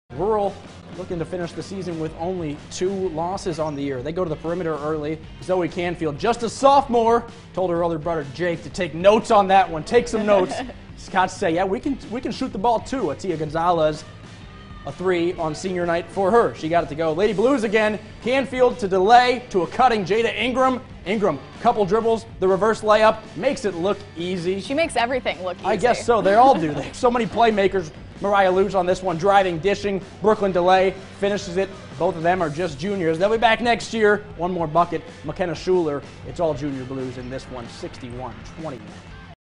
FULL COURT PRESS: Washburn Rural at Highland Park- Girls
Highlights from Washburn Rural's win over Highland Park in girls' basketball.